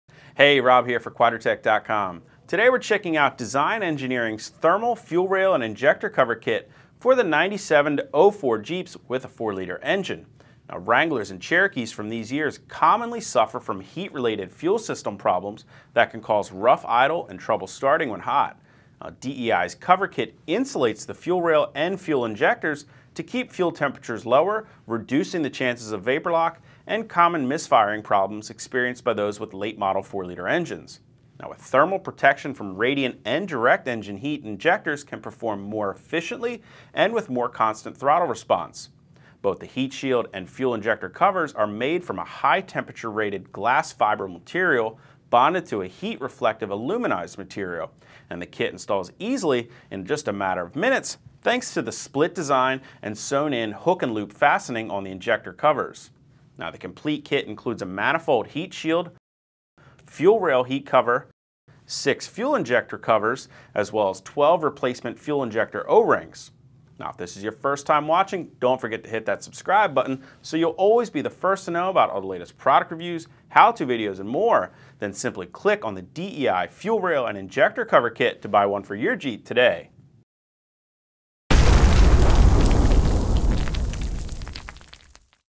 DEI Fuel Rail and Injector Cover Kit for Jeep Wrangler and Cherokee Review
Jeep Wranglers and Cherokees often are at risk of heat related fuel system problems that can cause rough idle or slow starting.
 DEI’s fuel rail and injector cover kit reduces rail and injector fuel temperatures; reducing instances of vapor lock.